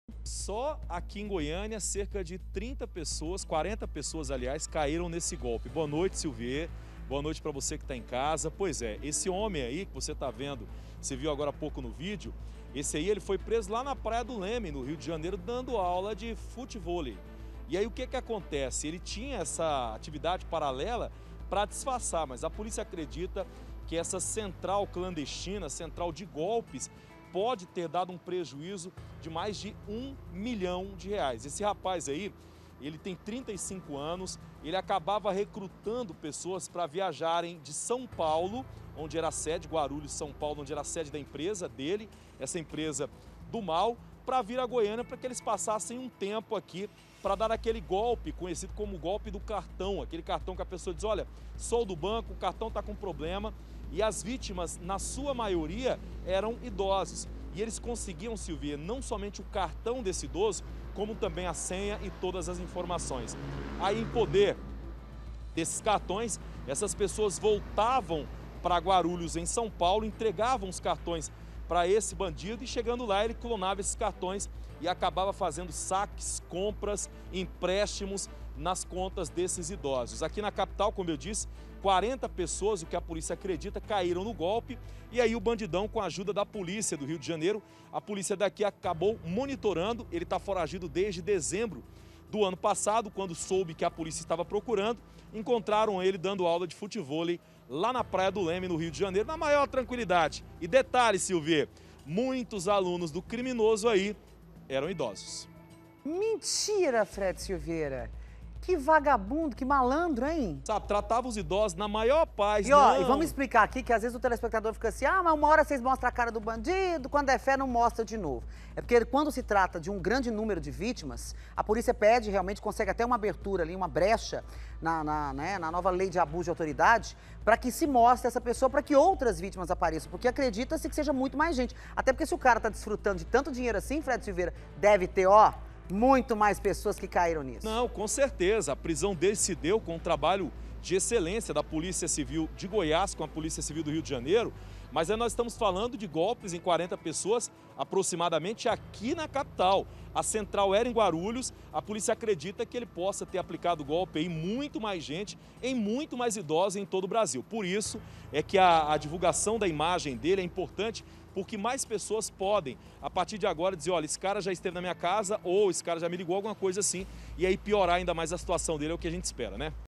ESTELIONATÁRIO PRESO: HOMEM MANTINHA FALSA CENTRAL TELEFÔNICA PARA APLICAR GOLPES EM IDOSOS
A QUE PONTO CHEGA A OUSADIA DOS BANDIDOS. UM HOMEM MONTOU UMA ESPÉCIE DE "EMPRESA" PARA DAR GOLPES EM IDOSOS. FRED SILVEIRA, ELE CHEGOU AO PONTO DE CRIAR UMA CENTRAL TELEFÔNICA SÓ PARA ENGANAR AS VÍTIMAS, É ISSO?